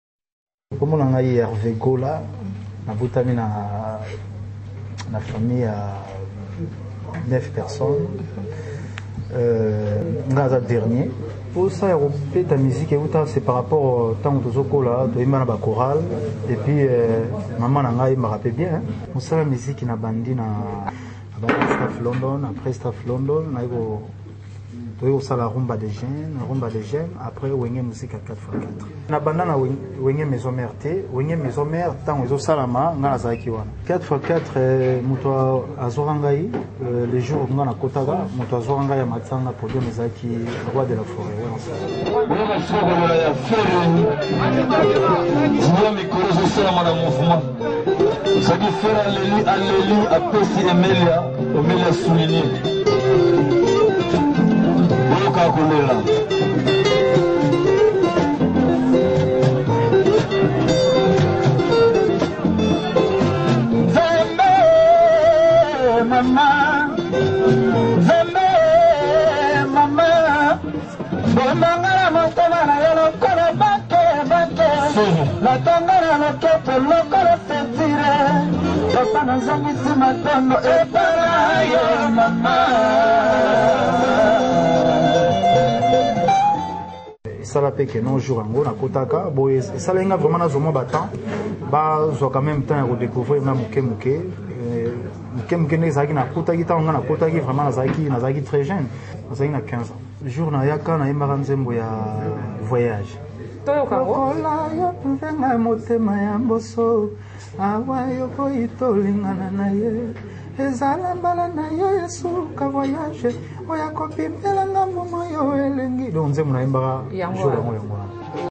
Ferre Gola dans WENGE BC BG 4*4
Ferre Gola teste dans wenge bcbg a l'age de 15 ans.